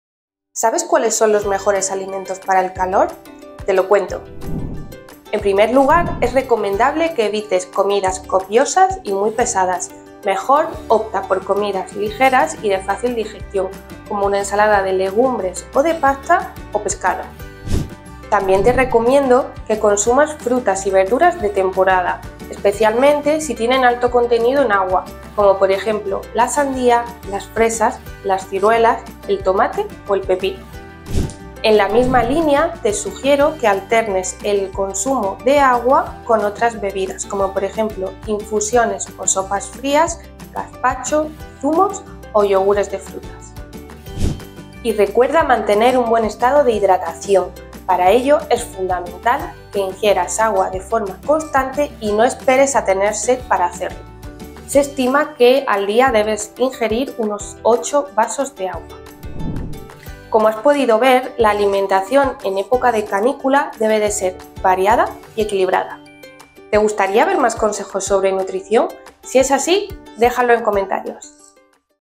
🍓 ¿Sabes cuáles son los mejores alimentos para el calor?
Una de nuestras especialistas te da algunos consejos de nutrición para estas fechas tan calurosas.